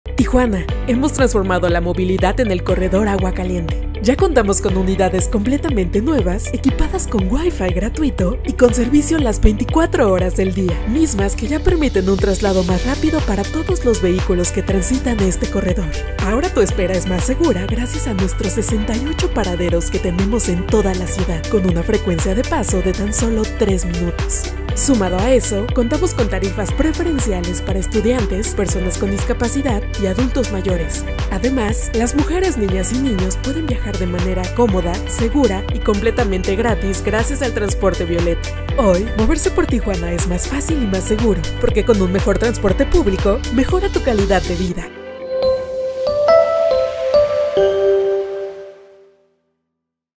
La nueva forma de Transitar el Corredor Agua Caliente
Unidades completamente nuevas, con WIFI gratuito y funcionando las 24 horas… Así Transformamos la movilidad en el Corredor Agua Caliente. 💚

¿Quién ya lo está utilizando? 🚌